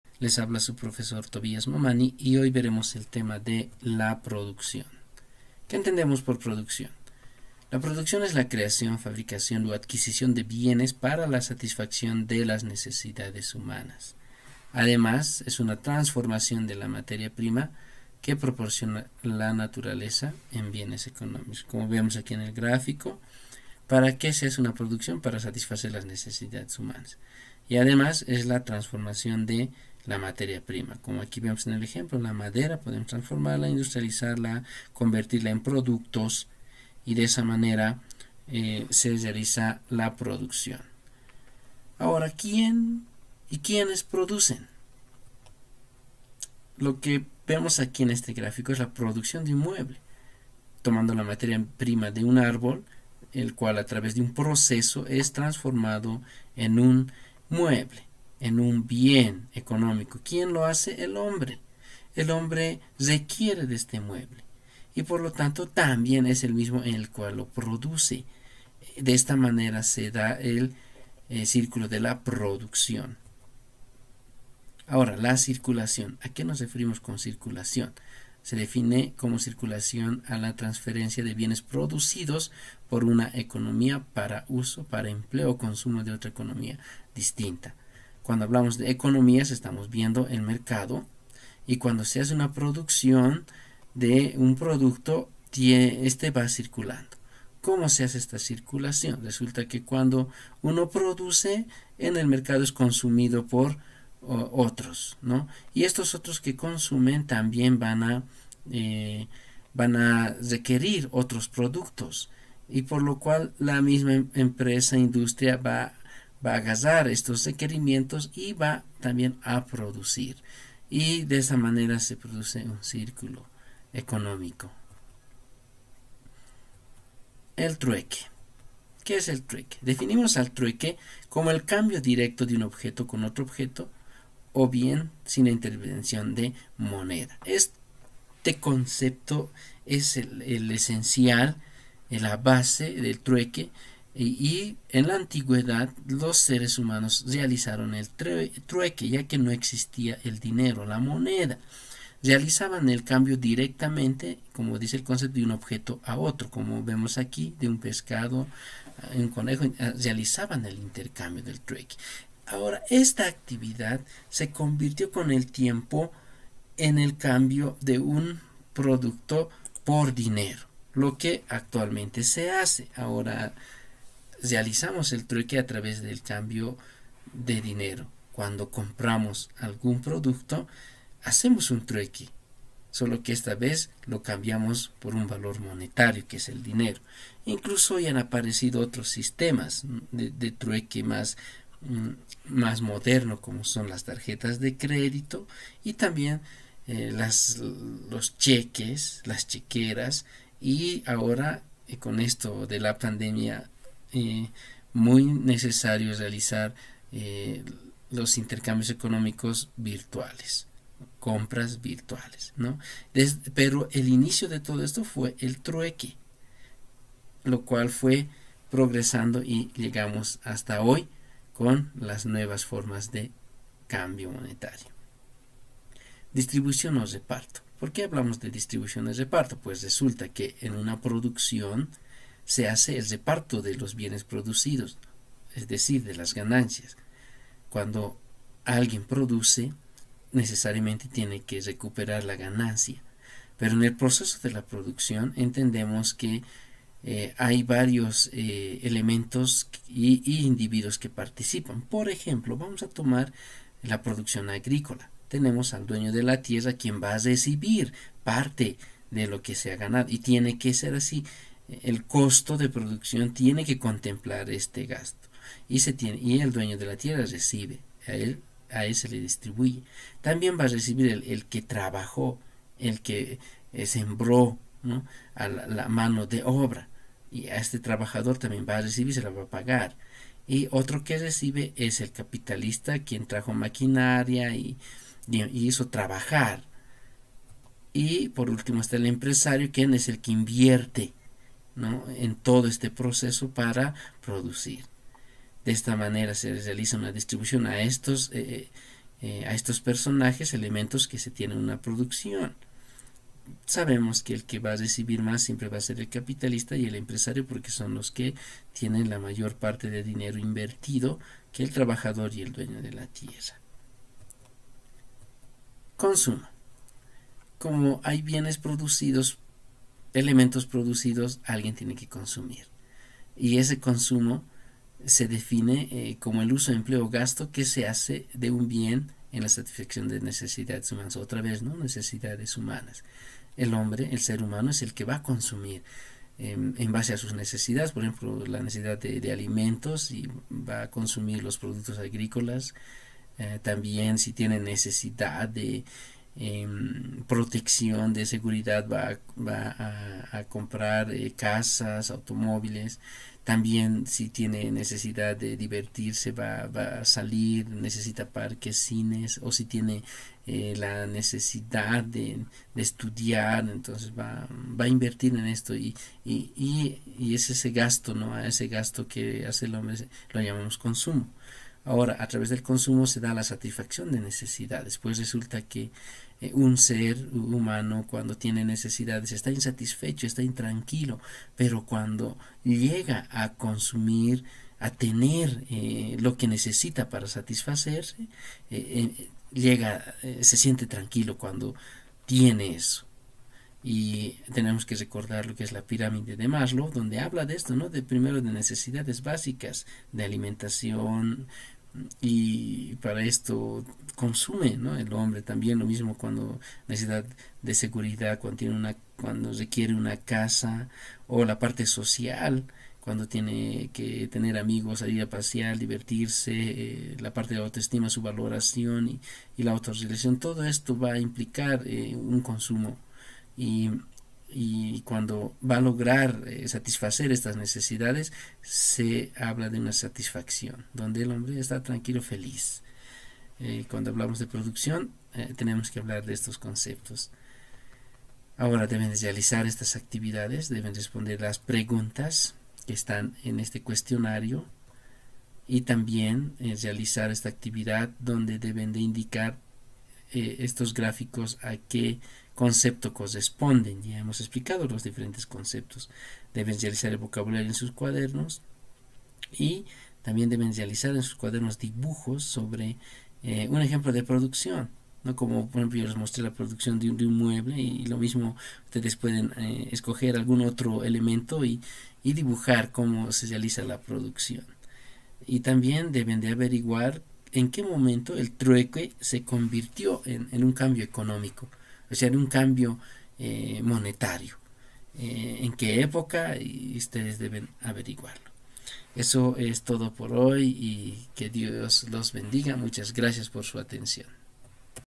LA PRODUCCIÓN1
EL TEMA DE LA PRODUCCIÓN SE EXPLICA BAJO LA PERSPECTIVA ECONÓMICA